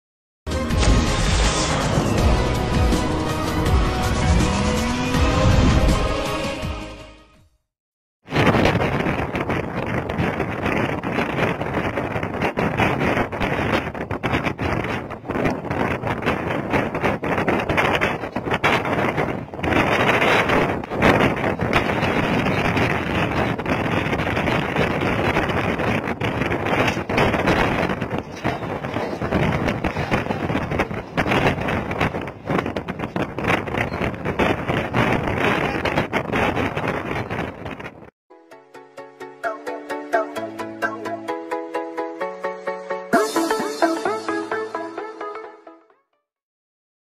Girl dangerous stunt failure in mumbai local train || lucky girl
Lucky girl local train stunt failure and saved by brave uncle